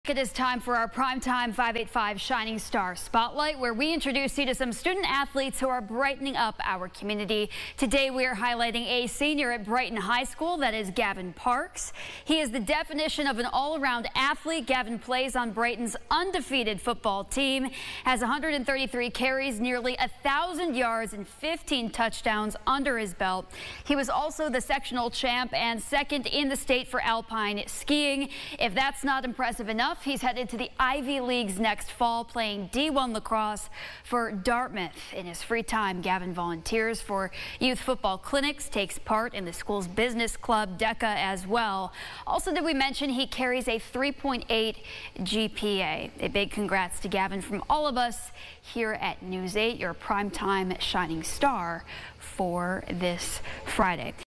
Primetime 585 Shining Star: Gavin Parks
Today's Primetime 585 Shining Star is Brighton High School senior Gavin Parks, the definition of an all-around athlete.